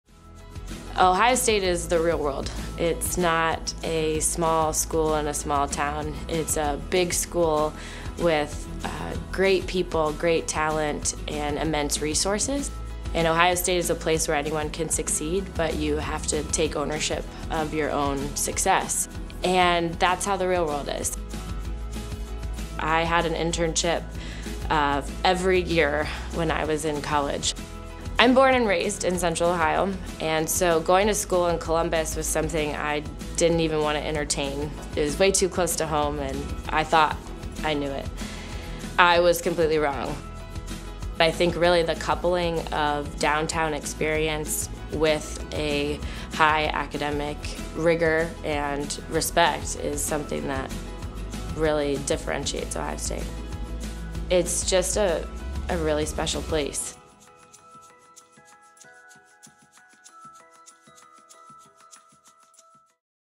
Ohio State Outcomes: Jordan Davis ’11
Being from Central Ohio, Ohio State was not at the top of Jordan's list of colleges to attend. She thought it was too close to home. But after discovering the opportunities and resources both Ohio State and Columbus had to offer, she knew this was the place to start her bright future.

Jordan earned degrees in Political Science and Leadership Studies from the College of Arts and Sciences. She is currently the manager of member activities and community affairs for The Columbus Partnership.